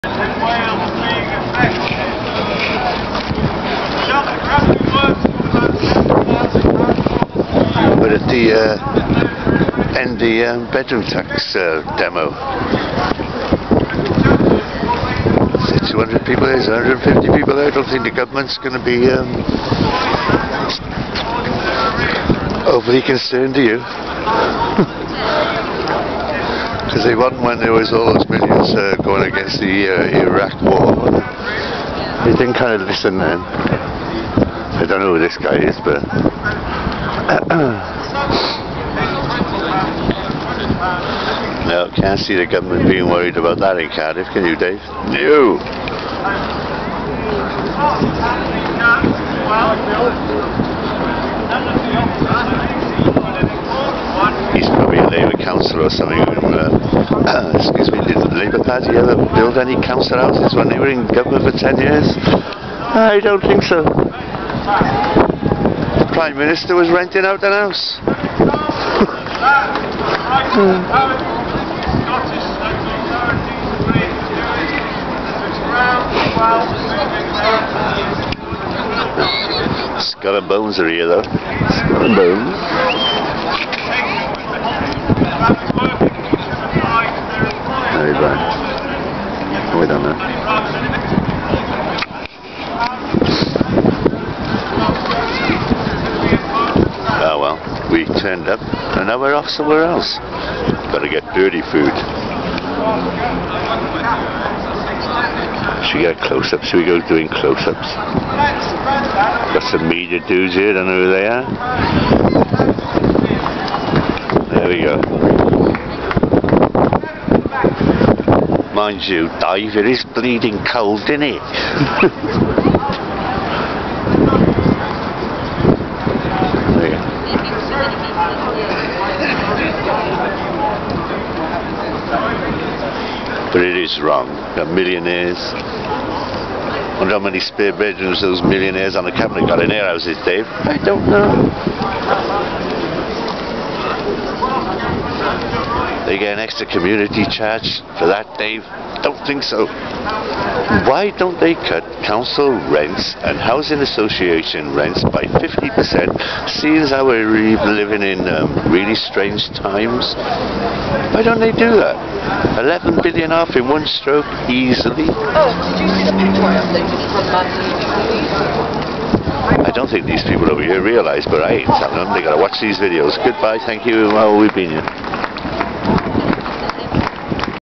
Stop The Bedroom Tax Demo Cardiff
for afull list of blogs. I don't think David Cameron and his crew are gonna be to frightened byt the amount of people turning out to this. Not when millions can attend Stop the War demos prior to going into Iraq and government of 10 people decide they wanna!